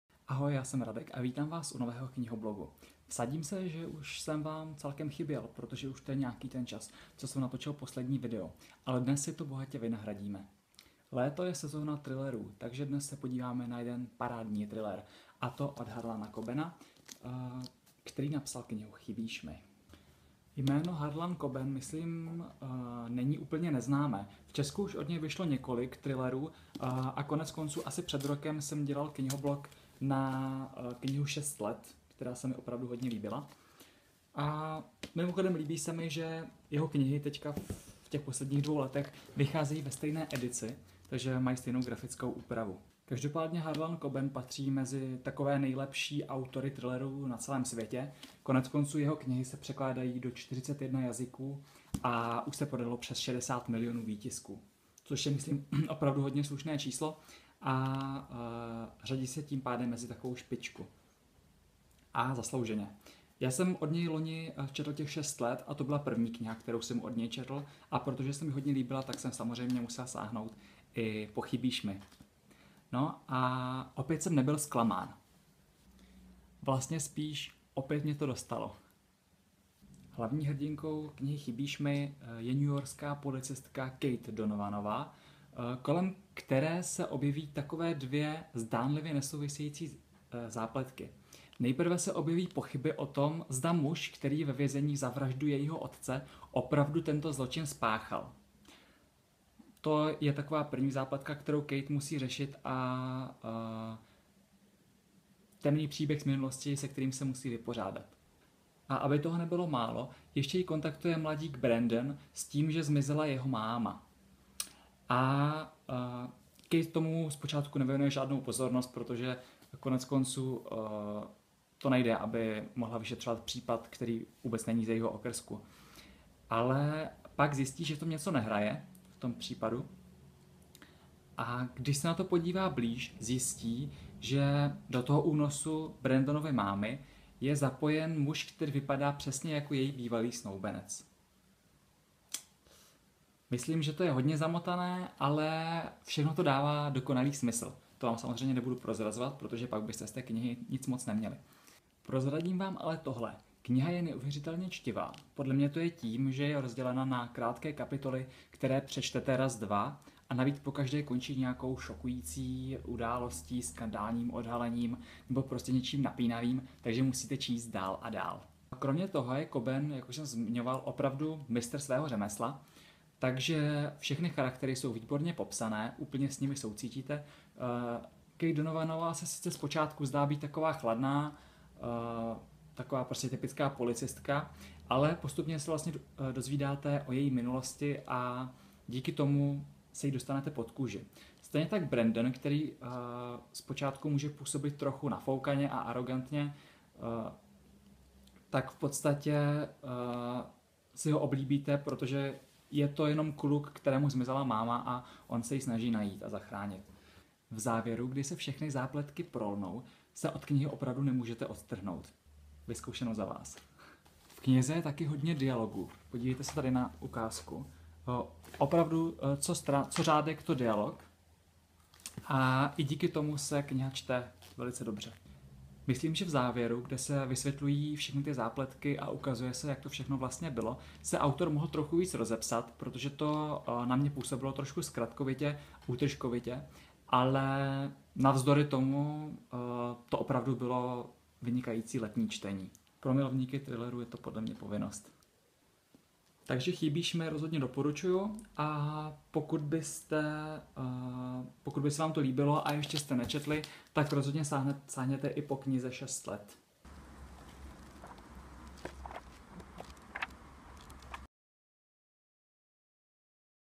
KNIHOBLOG: Chybíš mi (Harlan Coben)
KNIHOBLOG vás seznámí s knižními novinkami! Poslechněte si krátký úryvek, o čem kniha je a jak se mi (ne)líbila. Dnes recenzuji nejnovější thriller Harlana Cobena s názvem Chybíš mi (Missing You). Četli jste nebo se na něj taky chystáte? Napište mi do komentářů! :)

Další díly již brzy!